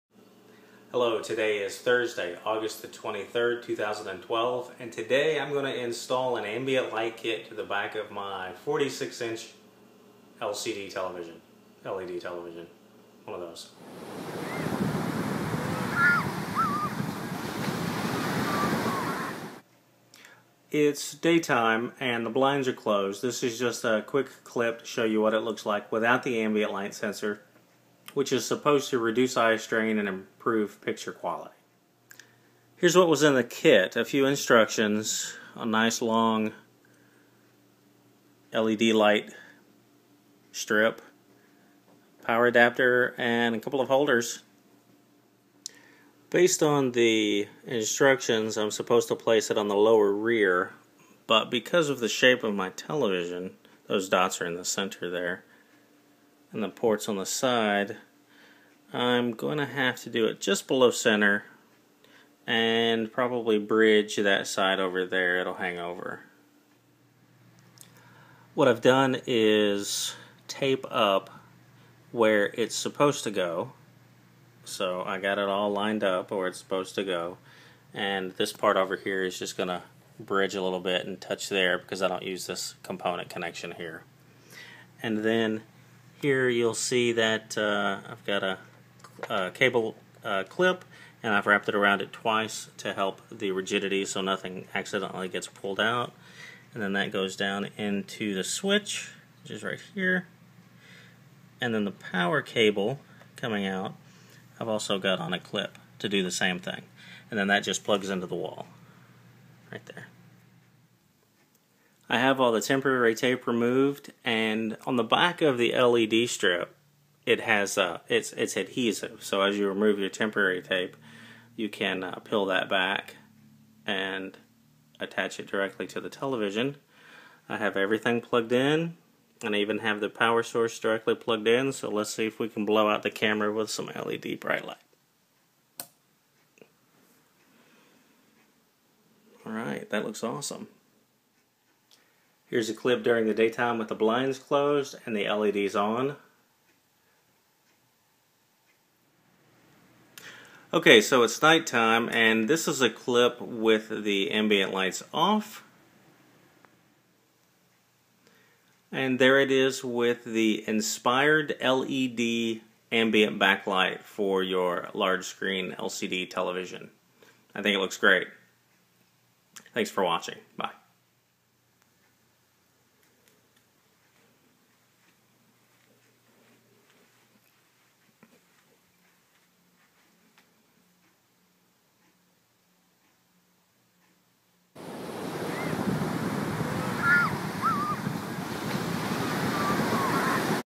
LED Backlight Kit Install for TV LCD Flatscreen - Video 231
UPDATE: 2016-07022

I install the Inspired LED Backlight Kit for 46"-57" TV LCD Flatscreen (Table Mount). I bought it from Amazon for $30. Install took less than 10 minutes, I could do another install in 5 minutes. I'm very happy with the results.

GET TO KNOW MORE ABOUT THE JONESES